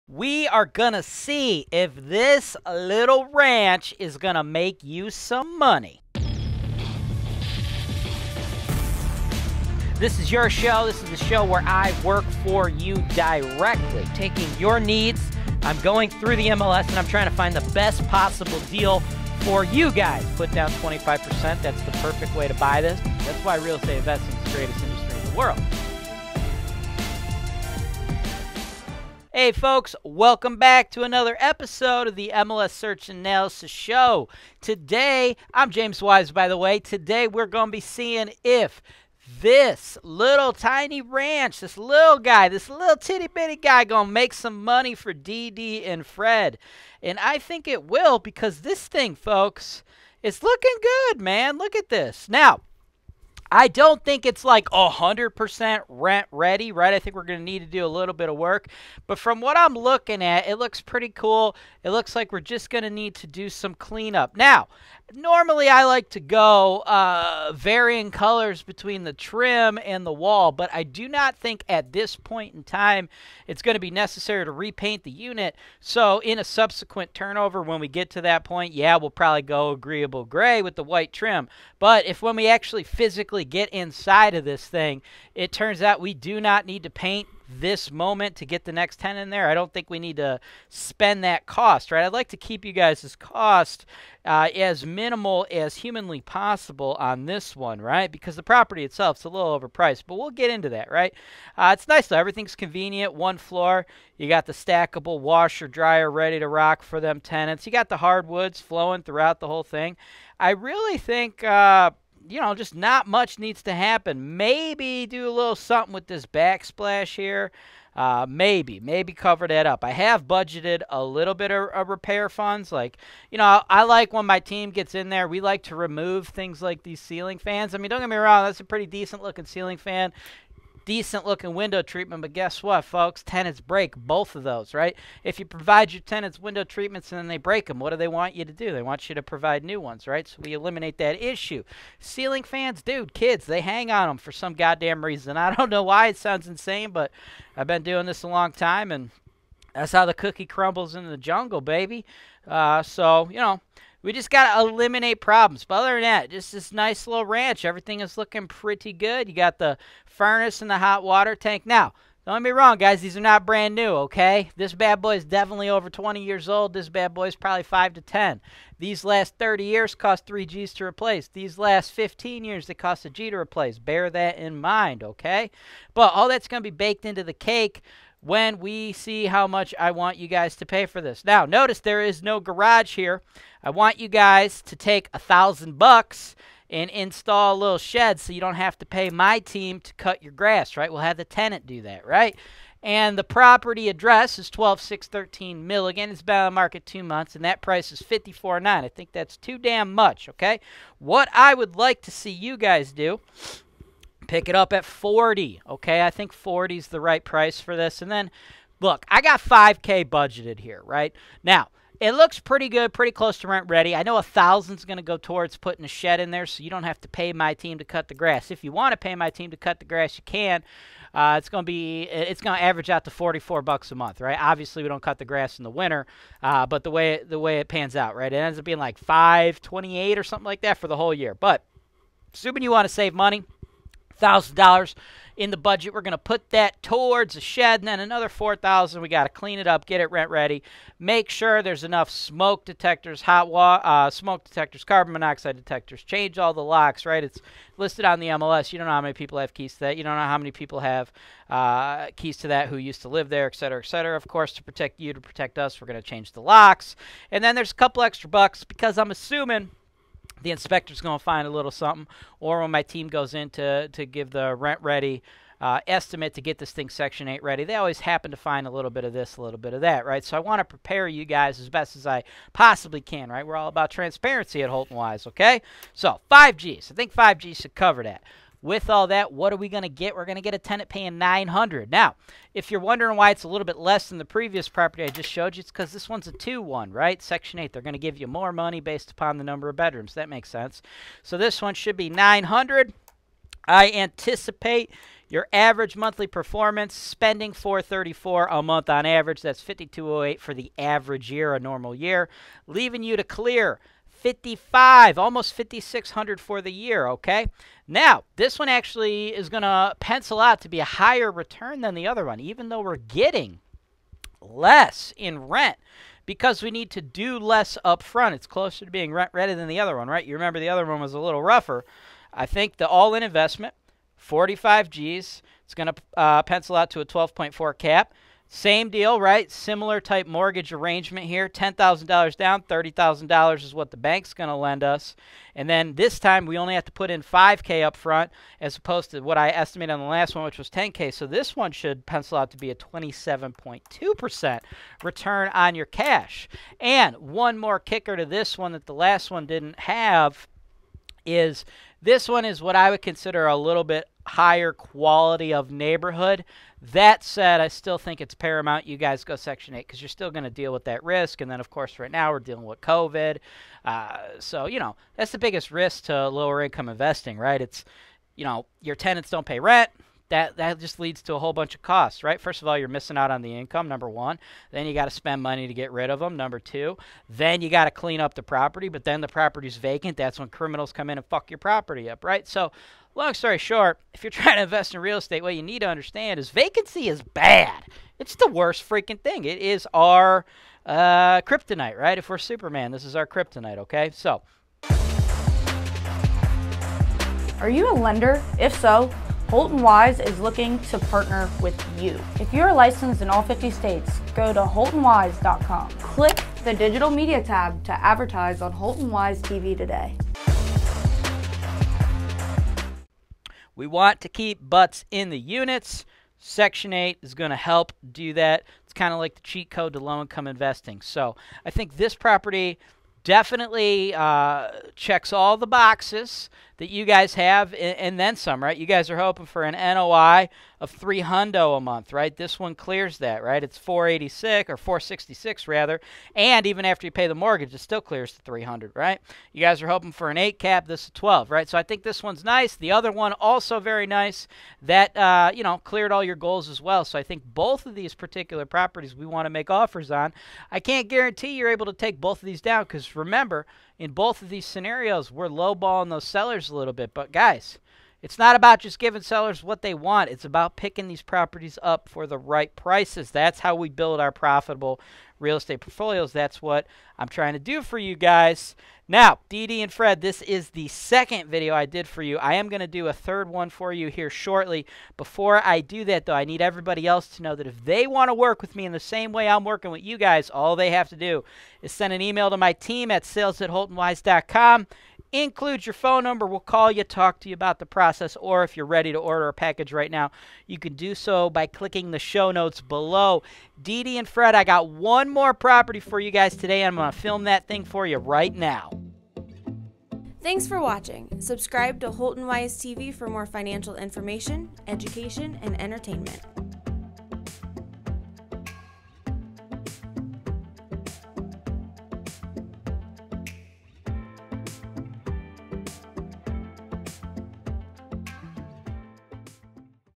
How to Analyze a Rental Property for Beginners | MLS Search & Analysis 313 - 12613 Milligan
The 313th episode of The MLS Search & Analysis Show is a real estate cash flow analysis for beginners. If you are new to real estate investing you will learn how to run the numbers on an out of state rental property with an easy to understand cash flow analysis performed by James Wise for real estate investors looking to get into long distance real estate investing. James goes over anticipated income, expenses, return on investment and ownership experience. Best of all this property can be purchased with Holton-Wise's turnkey services that include full service property management, maintenance and construction.

📺 HELPFUL RESOURCES
Holton-Wise FAQ